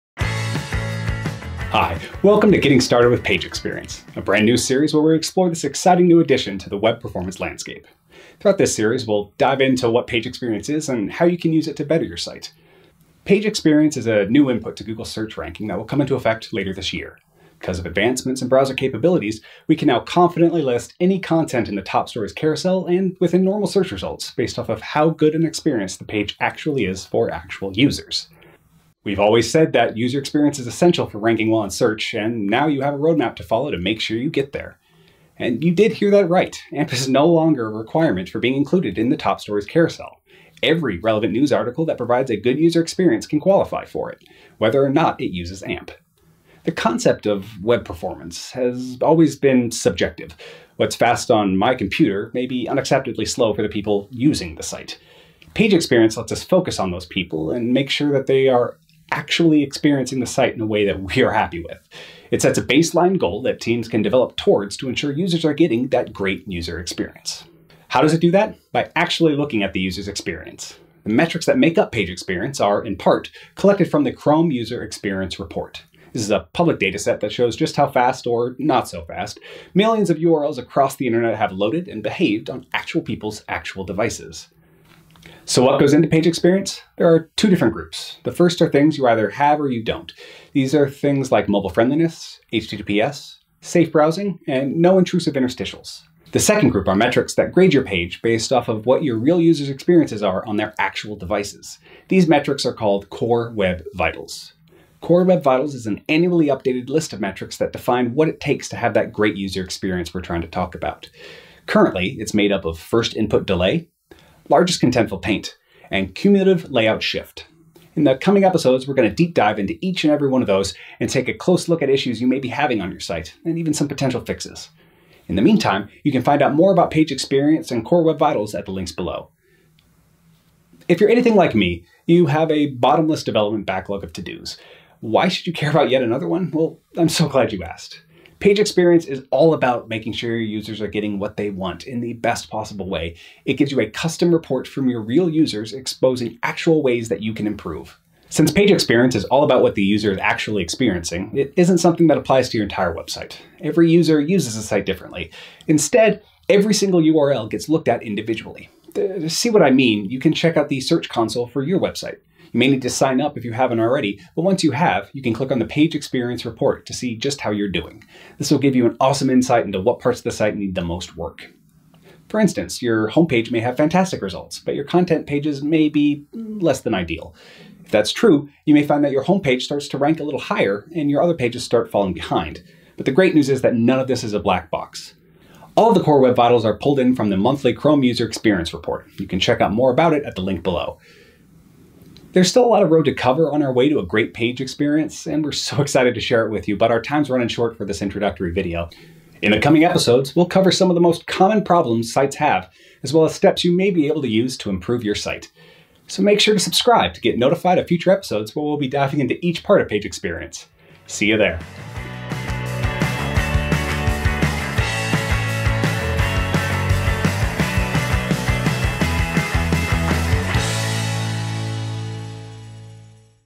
What goes into Page Experience
Page Experience is a new Google Search ranking factor that lays out clear guidelines for grading your users’ experience using Core Web Vitals. Learn more about how it’s measured, why it’s important, and what first steps to take next to make sure your users get the best possible experience on your web pages.